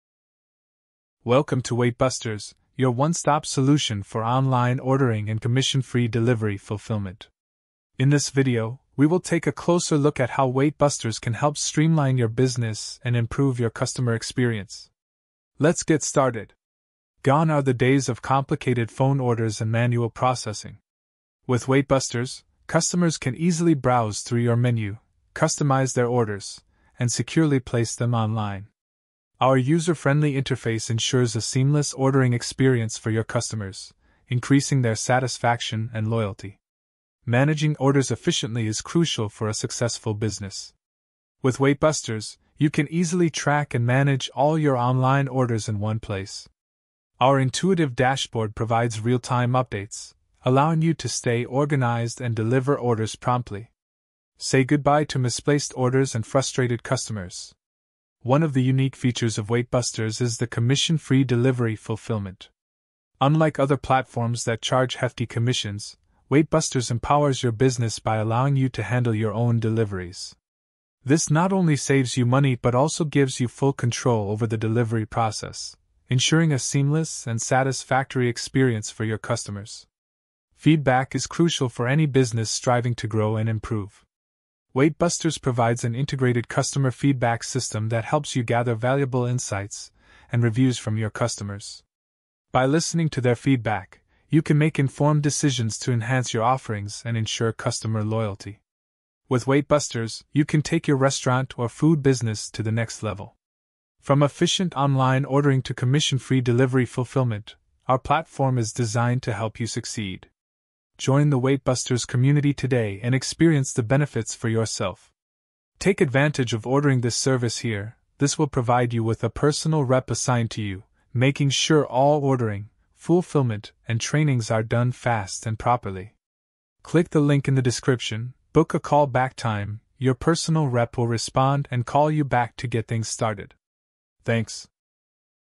Waitbusters | Online Ordering & Commission Free Delivery Fulfillment
xxxxxxxxxxxxxxxxxxxxxxxxxxxxxxxxxxxxxxxxxxxx
Welcome to WaitBusters, your ultimate solution for online ordering and commission-free delivery fulfillment. In this video, we will introduce you to the features and benefits of WaitBusters that can transform your restaurant or food business. Say goodbye to complicated phone orders and hefty commissions, and say hello to a streamlined ordering process and full control over your deliveries.

With WaitBusters, customers can easily browse through your menu, customize their orders, and securely place them online. Our user-friendly interface ensures a seamless ordering experience, increasing customer satisfaction and loyalty. You can easily manage all your online orders in one place with our intuitive dashboard, receiving real-time updates and staying organized for prompt delivery.

One of the standout features of WaitBusters is the commission-free delivery fulfillment. Unlike other platforms that charge high commissions, WaitBusters allows you to handle your own deliveries. This not only saves you money but also gives you full control over the delivery process, ensuring a seamless and satisfactory experience for your customers. Join the WaitBusters community today and take your restaurant or food business to new heights!

WaitBusters, online ordering, commission-free delivery fulfillment, restaurant, food business, customer satisfaction, streamlined ordering process, intuitive dashboard, control over deliveries, customer loyalty

Key Moments:
00:00:00 Introduction to Weight Busters
00:00:07 Benefits of Weight Busters
00:00:13 User-friendly interface
00:00:19 Streamlining orders and menu customization
00:00:26 Secure online ordering
00:00:33 Efficient order management
00:00:38 Track and manage online orders
00:00:43 Intuitive dashboard for real-time updates
00:00:47 Commission-free delivery fulfillment
00:00:51 Handling your own deliveries
00:00:56 Feedback system for customer insights
00:01:01 Enhancing offerings based on feedback
00:01:05 Taking your restaurant to the next level
00:01:10 Benefits of Weight Busters
00:01:15 Joining the Weight Busters Community
00:02:09 Booking a call back for assistance